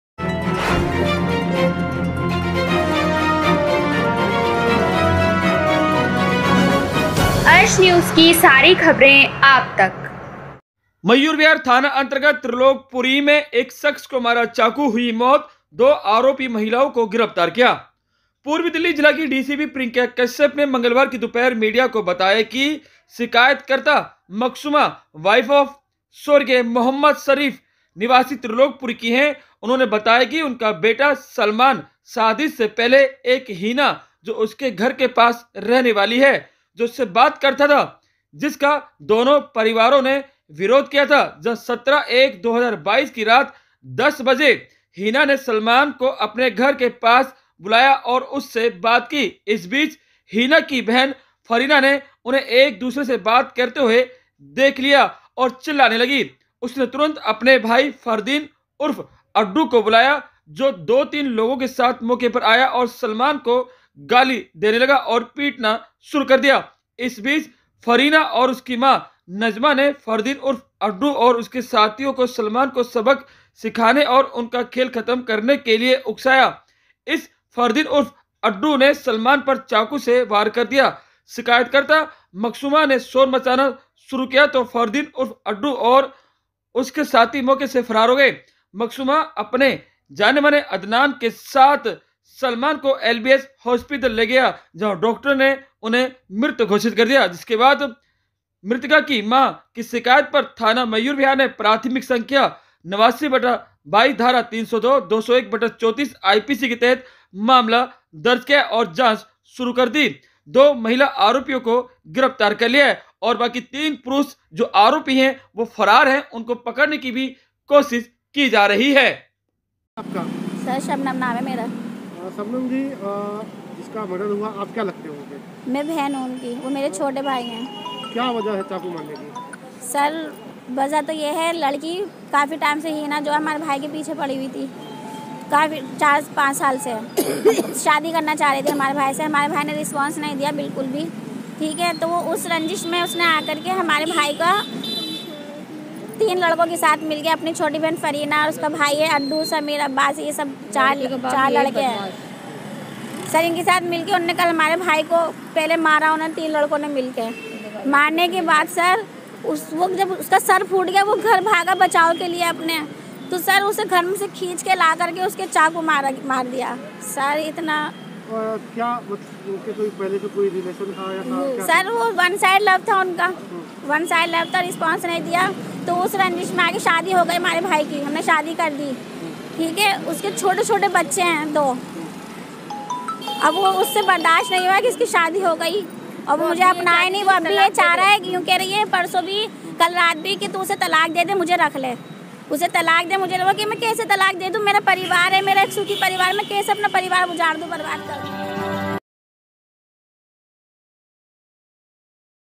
त्रिलोकपुरी में एक शख्स को मारा चाकू हुई मौत दो आरोपी महिलाओं को गिरफ्तार किया
मयूर विहार थाना अंतर्गत त्रिलोकपुरी में एक शख्स को मारा चाकू हुई मौत दो आरोपी महिलाओं को गिरफ्तार किया